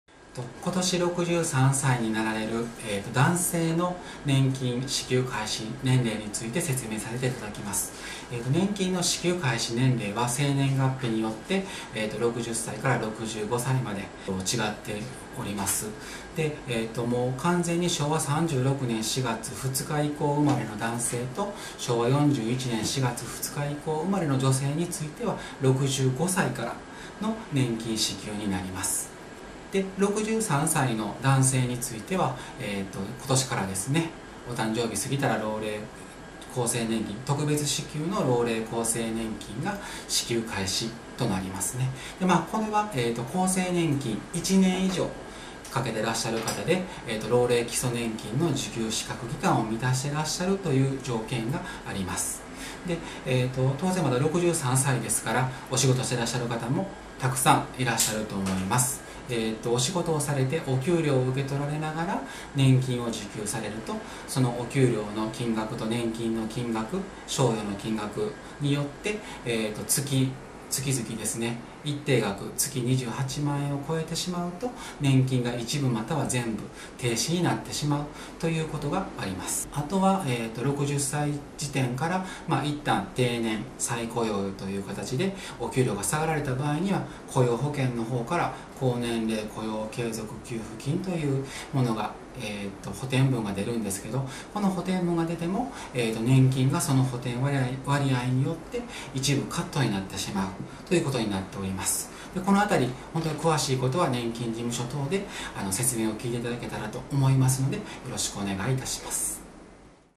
年金教えて！社労士さん（２）特別支給の老齢厚生年金て何？
６３歳になる会社員男性Ｂさんが社労士事務所を訪れて尋ねました。「６５歳からじゃなくて今年から年金がもらえるって聞いたのですが？」。支給年齢の変更による移行措置があるそうで、その内容は？